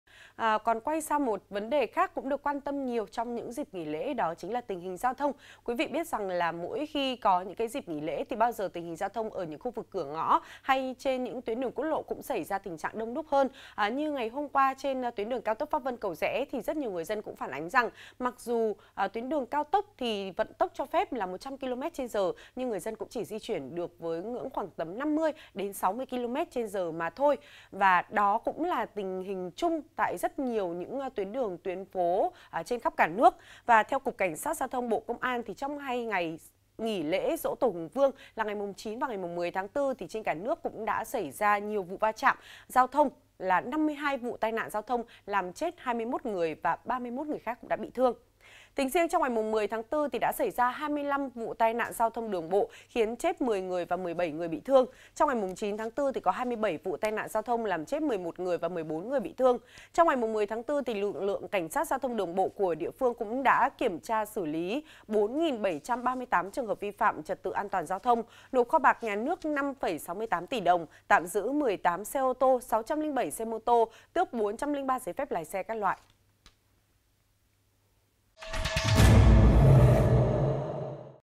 52 NGƯỜI THƯƠNG VONG DO TAI NẠN GIAO THÔNG TRONG 2 NGÀY NGHỈ LỄ GIỖ TỔ | VTC9
VTC9 | 52 NGƯỜI THƯƠNG VONG DO TAI NẠN GIAO THÔNG TRONG 2 NGÀY NGHỈ LỄ GIỖ TỔ